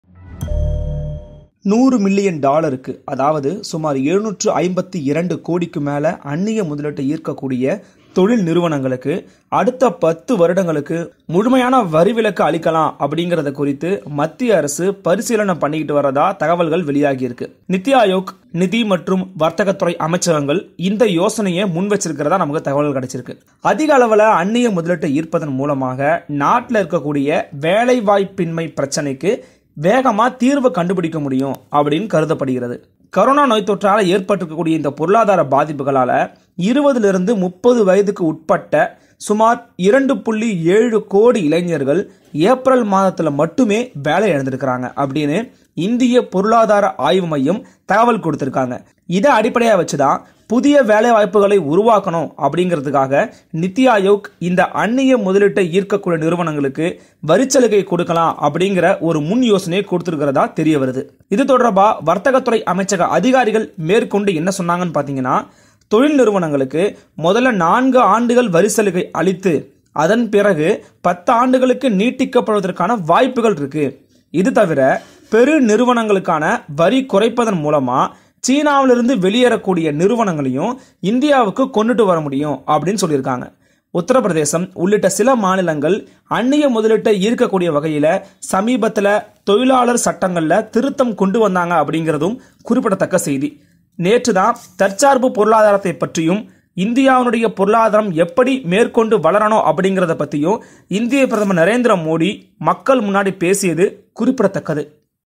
2.7 கோடி பேர் ஏப்ரல் மாதத்தில் வேலையிழப்பு | இந்திய பொருளாதார ஆய்வு மையம் | Aadhan Tamil
2.7 கோடி பேர் ஏப்ரல் மாதத்தில் வேலையிழப்பு | இந்திய பொருளாதார ஆய்வு மையம் | Aadhan Tamil